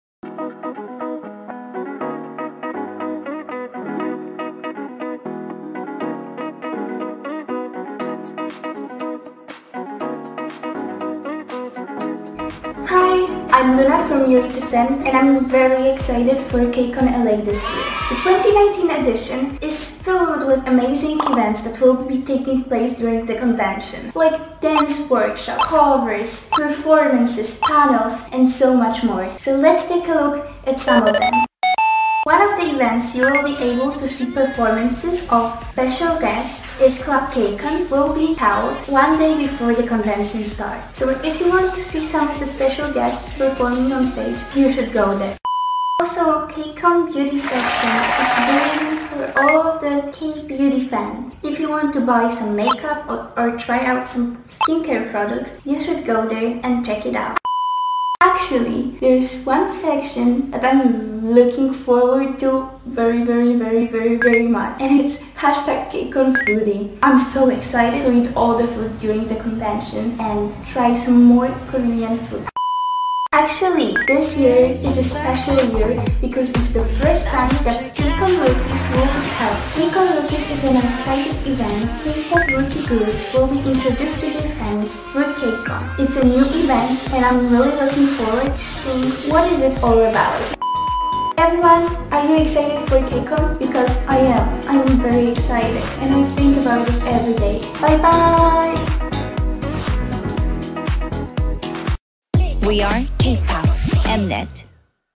[KCON19LA] What′s new′19?
Have you heard? Newest additions were made to KCON2019LA

Listen to our one and only UHSN's LUNA in order to get familiar with what's new this year and enjoy it to the fullest